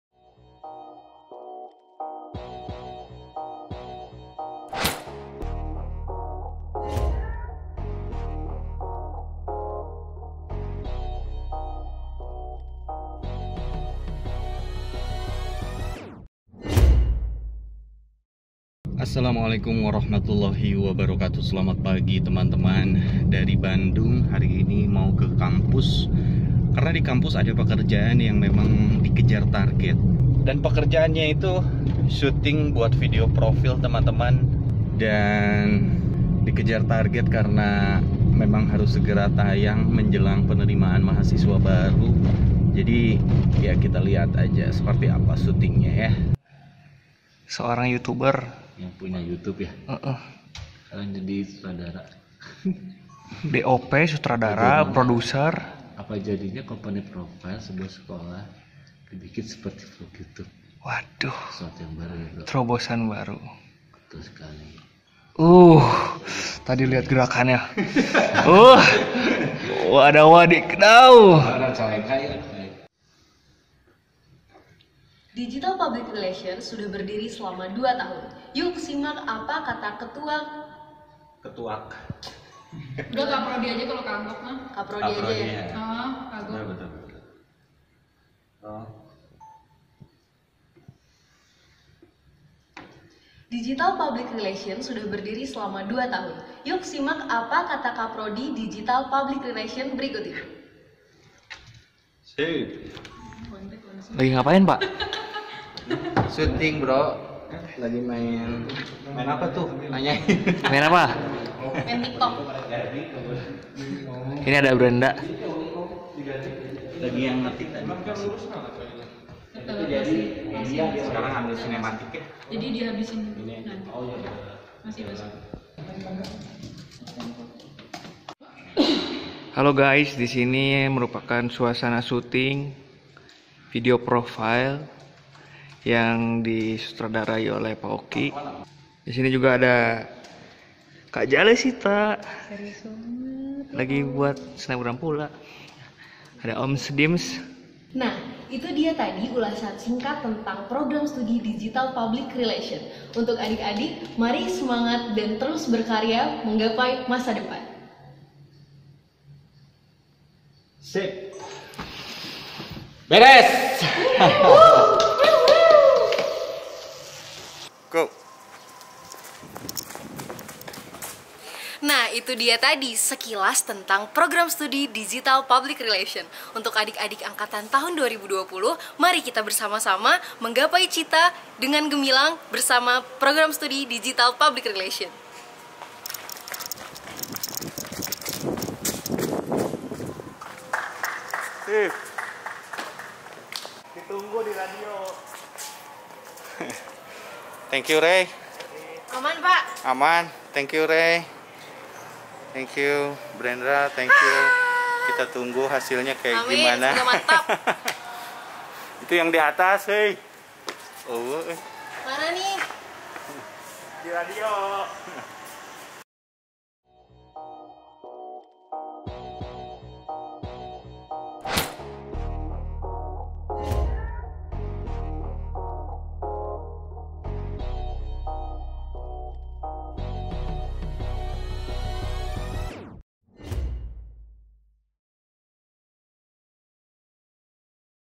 B Roll Film Bareng Finalis INDONESIAN IDOL Brenda Qaulani
Bikin Video B roll dengan talent Finalis INDONESIAN IDOL, seru, menyenangkan dan tidak banyak retake, alhamdulillah semua berjalan lancar.